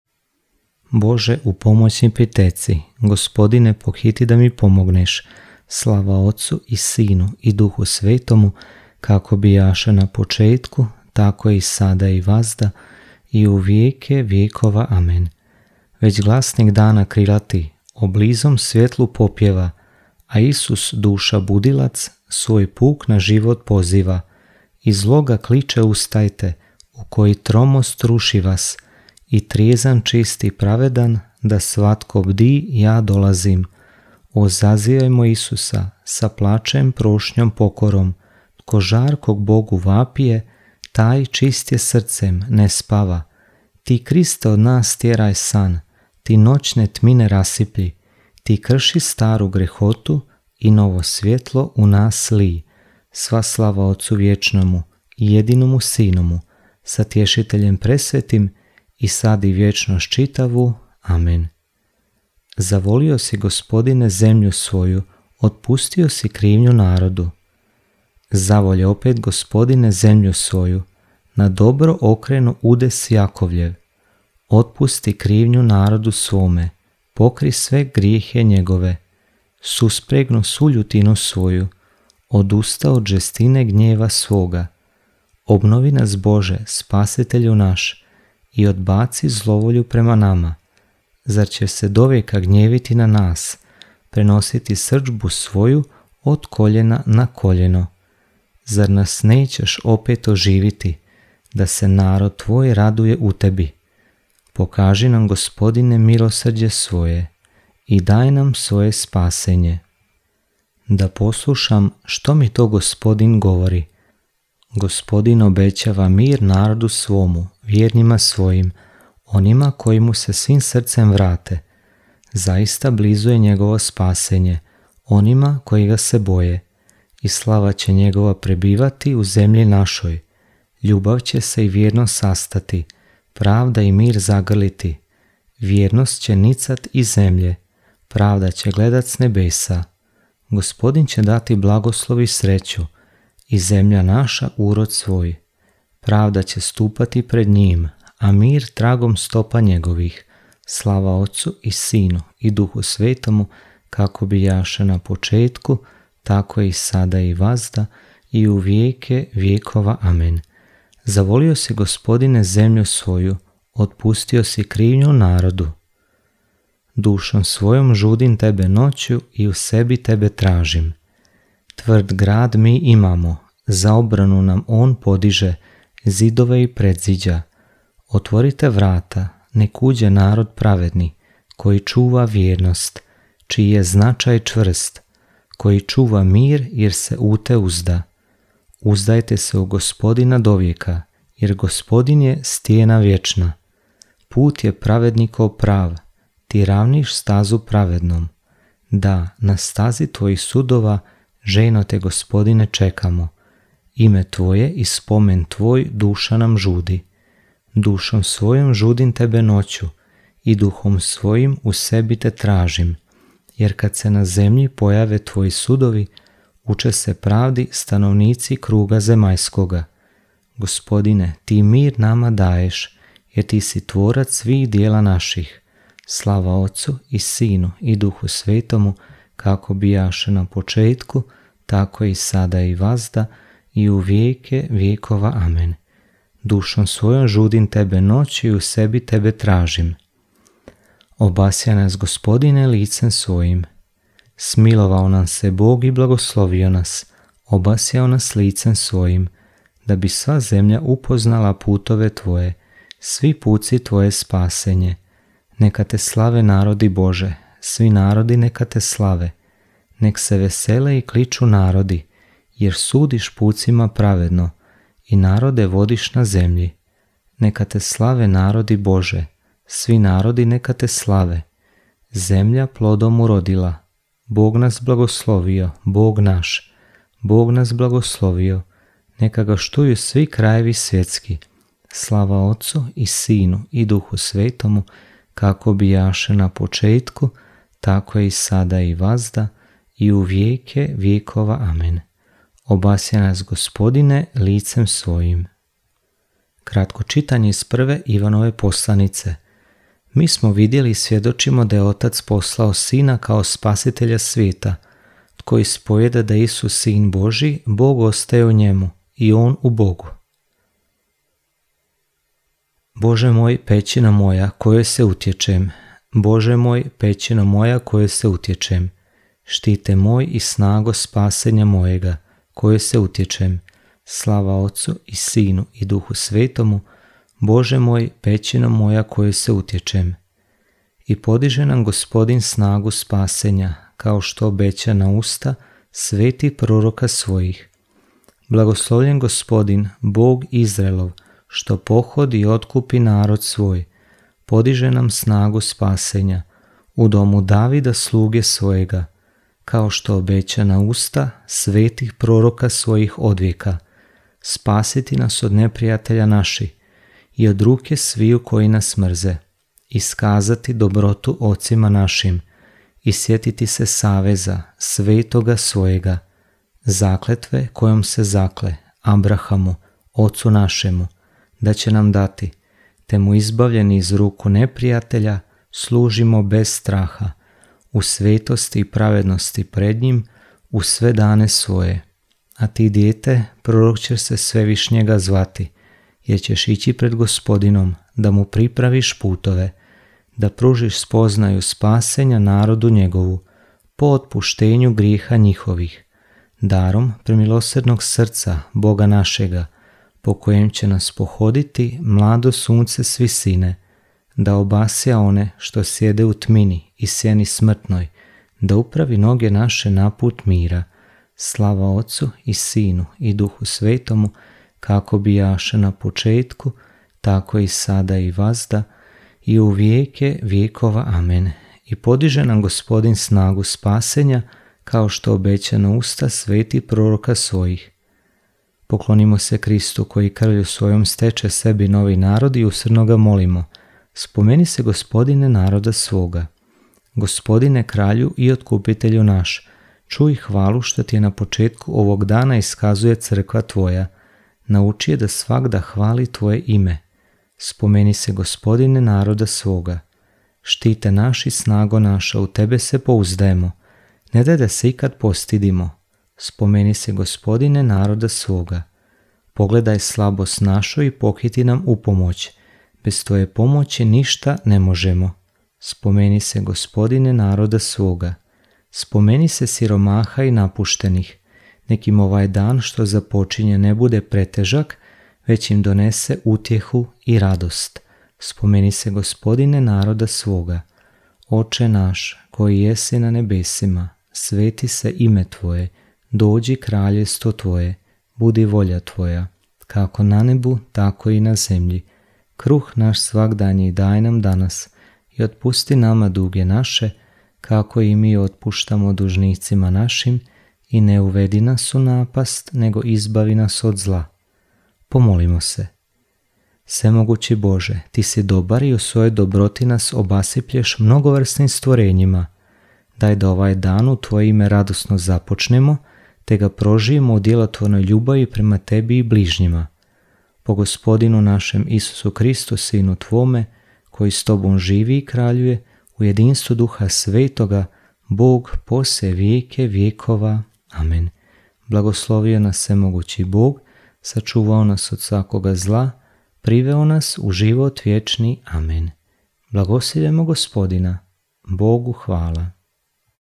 Časoslov: JUTARNJA MOLITVA - UTORAK 13.8.2024.
Jutarnja molitva iz časoslova koja se moli u utorak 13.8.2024.

Svećenici, redovnici i đakoni obvezni su dnevno moliti tzv. liturgiju časova, božanski časoslov ili brevijar. Sastoji se od skupine molitava za svaki dan, u različitim trenucima, od jutra do noći. To je molitvena rutina koja nije rezervirana za Bogu posvećene osobe, nego se preporučuje i laicima u Crkvi.

Ovo su dnevne molitve liturgije časova: Jutarnja molitva ili Laude – Pohvale (moli se u zoru ili u rano jutro) Služba čitanja (može se kombinirati s drugom molitvom u toku dana) Srednji čas (koji se može moliti između Jutarnje i Večernje molitve) Treći čas (moli se oko 9 sati ujutro) Šesti čas (moli se oko podneva) Deveti čas (moli se popodne, oko 15 sati) Večernja molitva ili Vespere (moli se predvečer, oko 18 sati) Povečerje (moli se prije počinka, obično u 20 ili 21 sat)